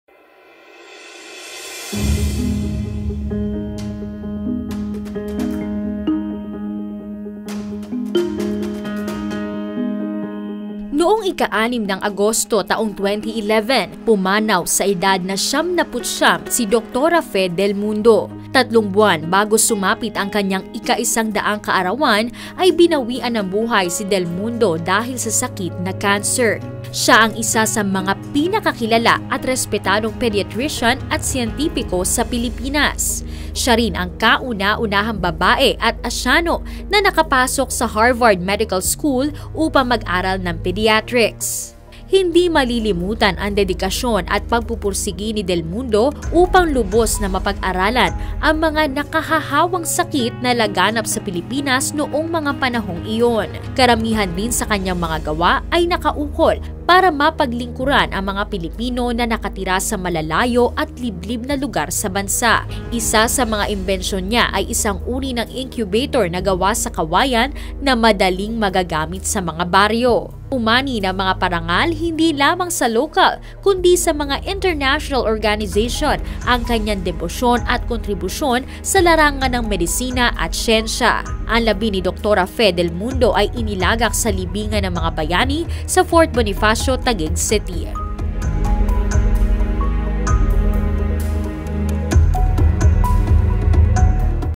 Death anniversary of Dr. Fe del Mundo, a respected Filipino pediatrician and scientist
Dr. Fe del Mundo, a known and well-respected pediatrician and scientist in the Philippines died on August 6, 2011. Her devotion and contributions in the field of medicine received recognition from international organizations.